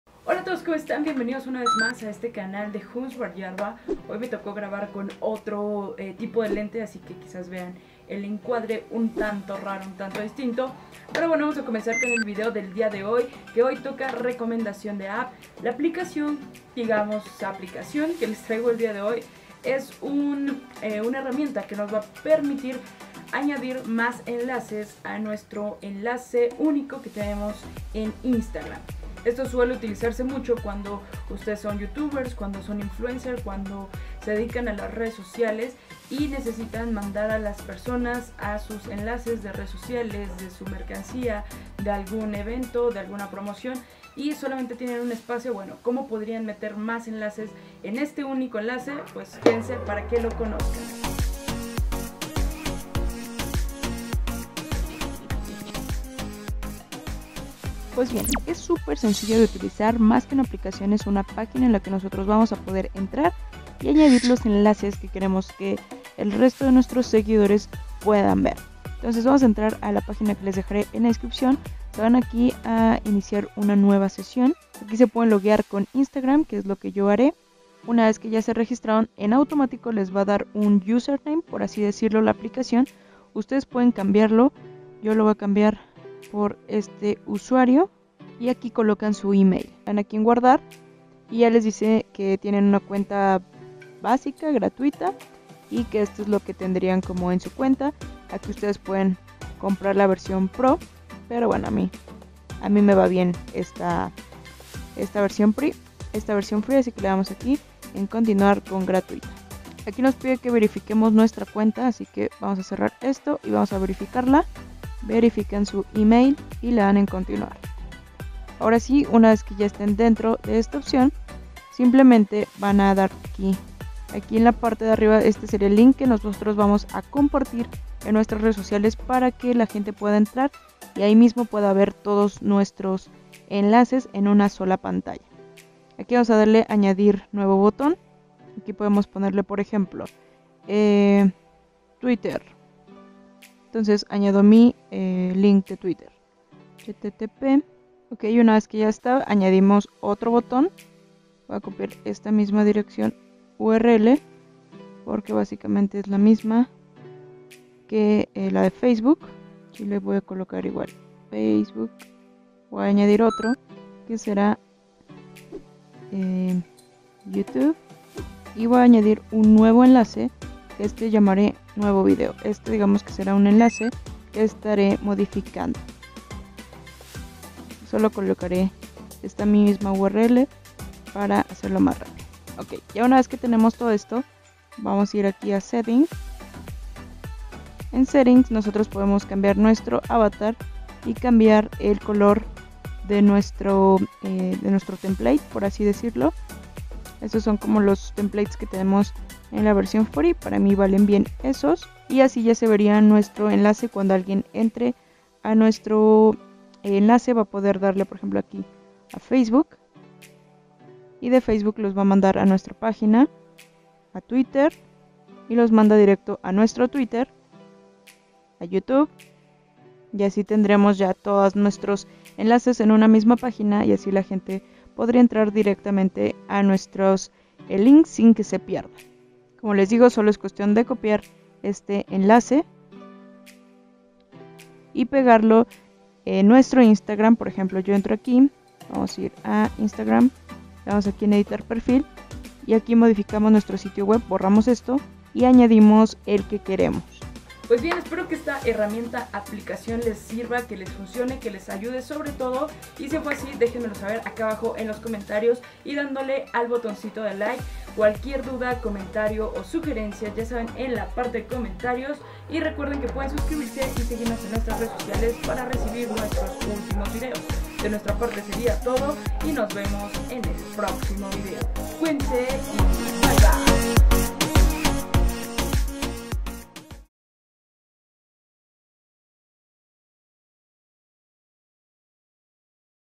PONER MÁS DE UN ENLACE EN BIOGRAFIA DE INSTAGRAM | GRATIS
EN ESTE VIDEO LES MUESTRO UNA FORMA SENCILLA Y GRATUITA DE PONER MÁS DE UN ENLACE EN NUESTRO LINK DE LA BIOGRAFÍA DE INSTAGRAM